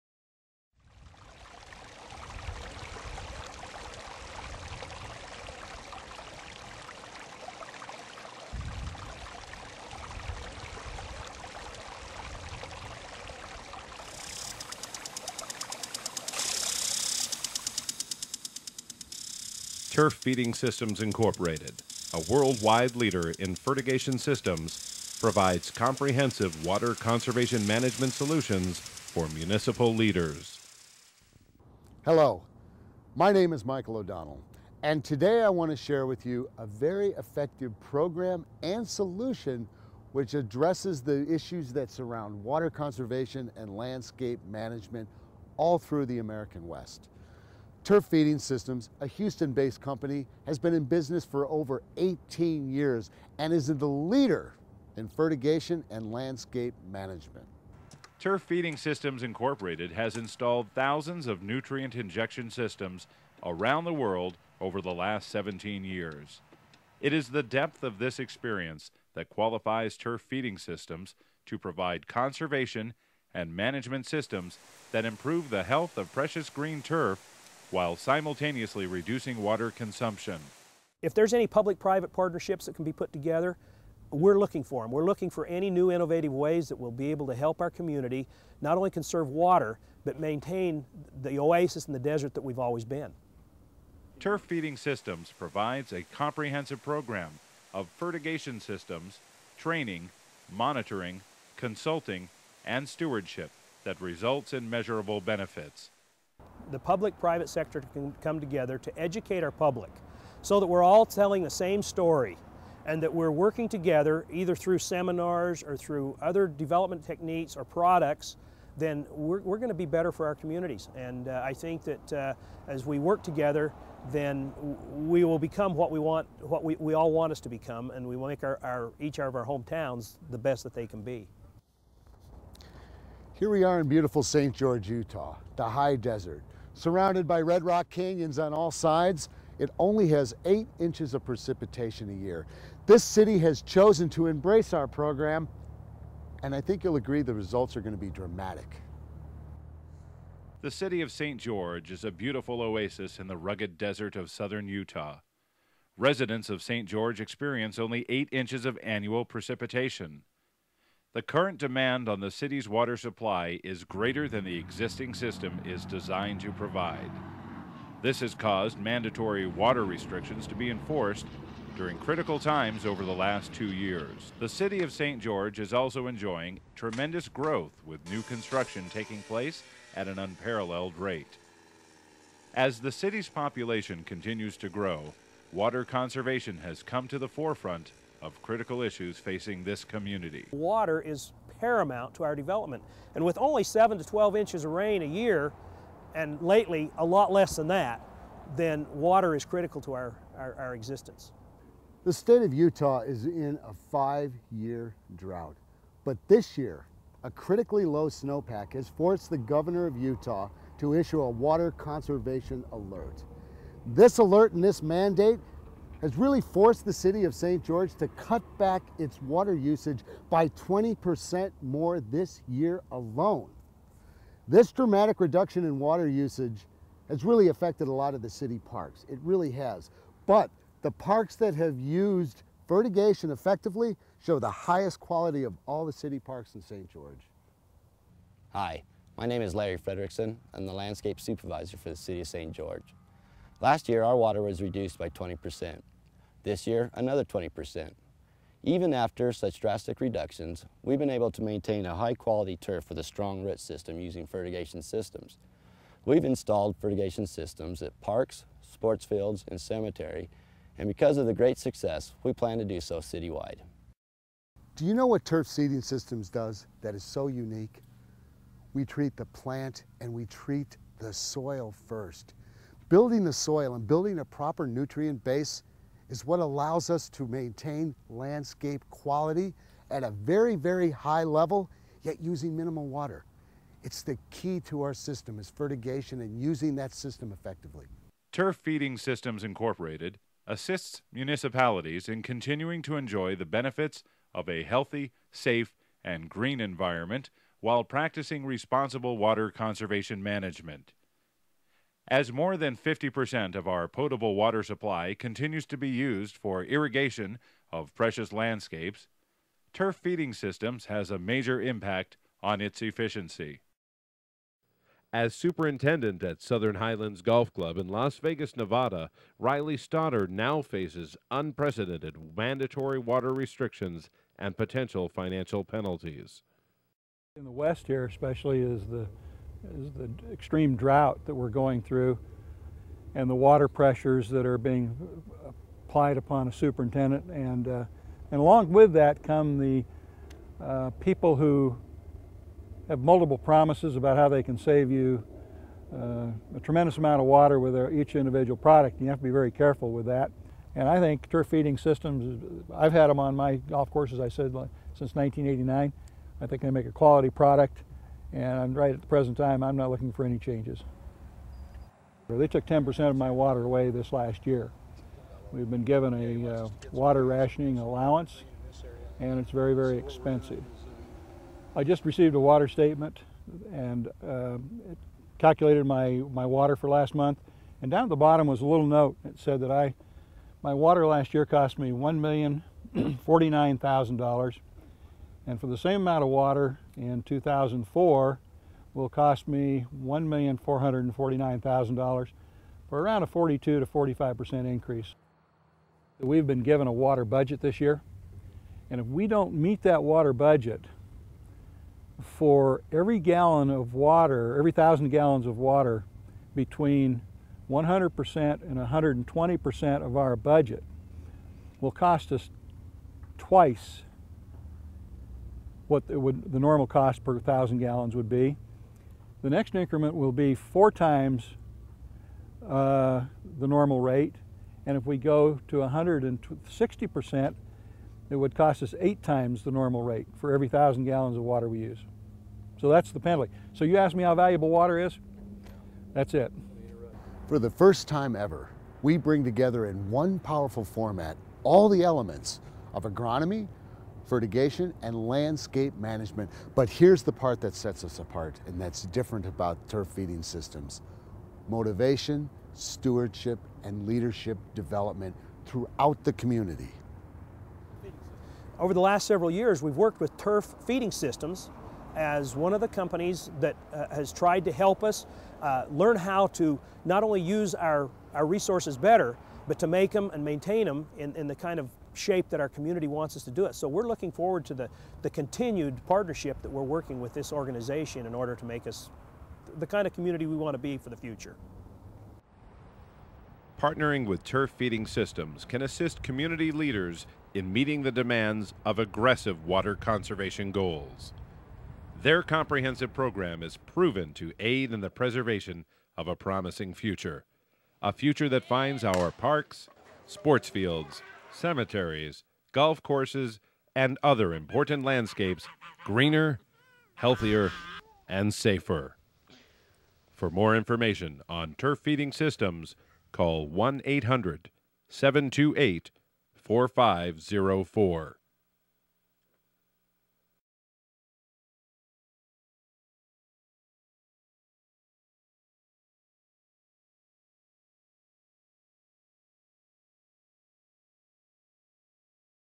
Water Conservation Through Turf Feeding Systems
Turf Feeding Systems highlights the importance of water conservation.